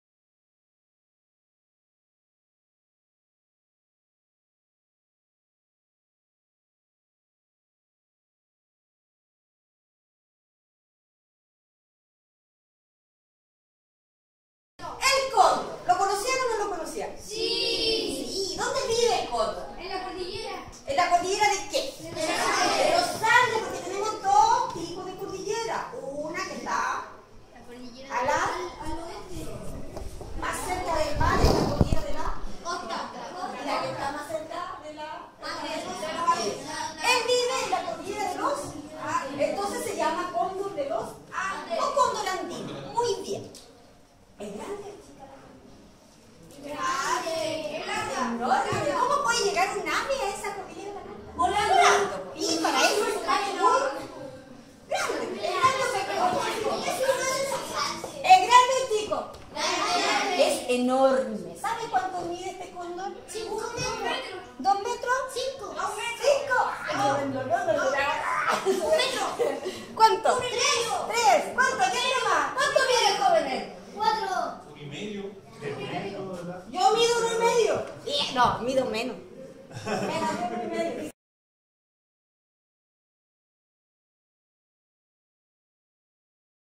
Visita Escuela El Patagual.
Convenio de educación ambiental entre la Corporación Nacional Forestal Conaf y el Museo de Historia Natural de Valparaíso.